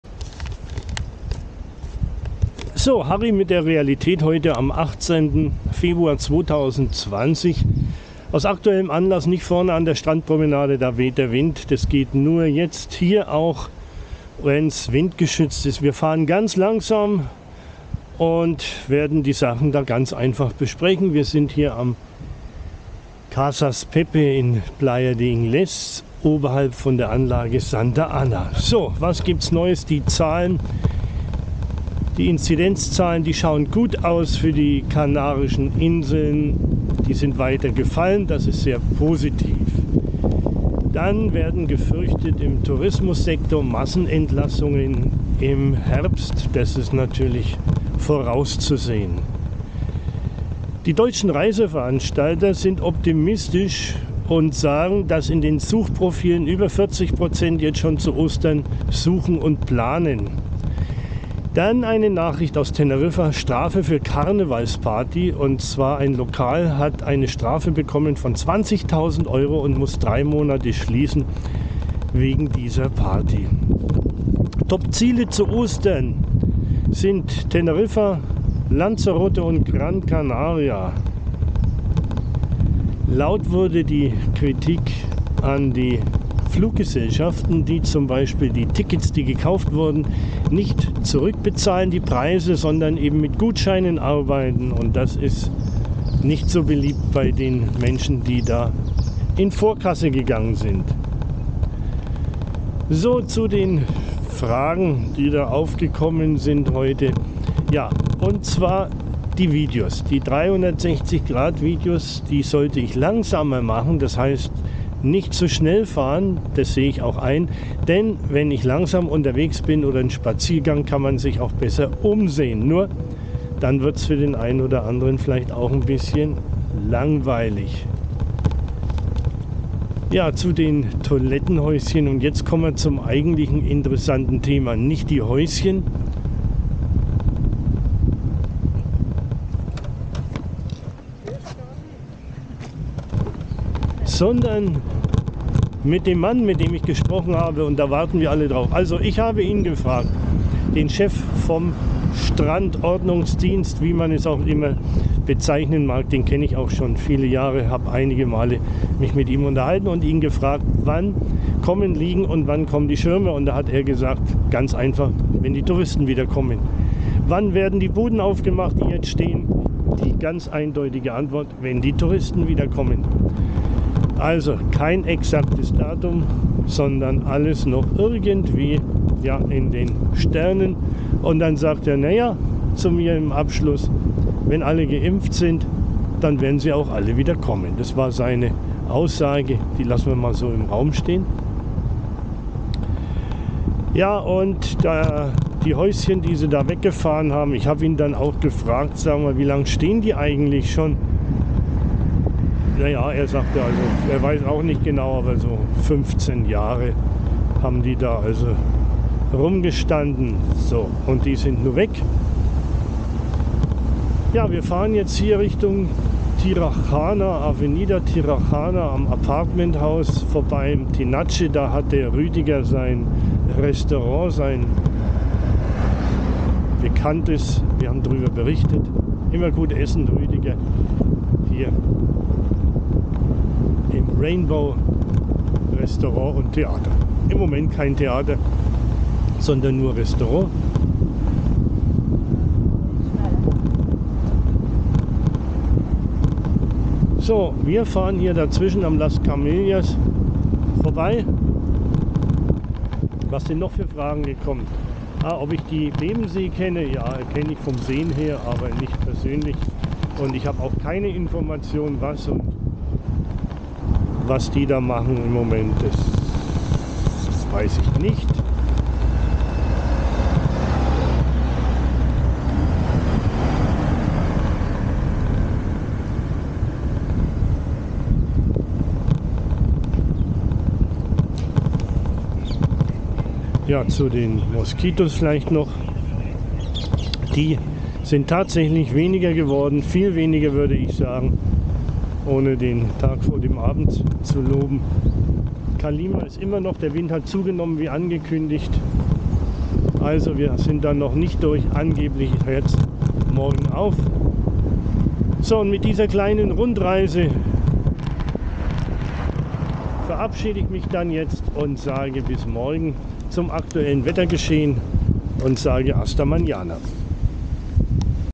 Realität Gran Canaria 18.2.21 ....ab Montag evt. Gran Canaria Stufe 2...am Sonntag wissen wir mehr..
Die aktuellen Corona Zahlen .
Topziele zur Osterwoche.
Deutsche Reiseveranstalter sind optimistisch.
Kritik an den Fluggesellschaften.
Markenfälschung bei Routinekontrolle entdeckt.

Video ohne  Kommentar
Danke schön

Gracias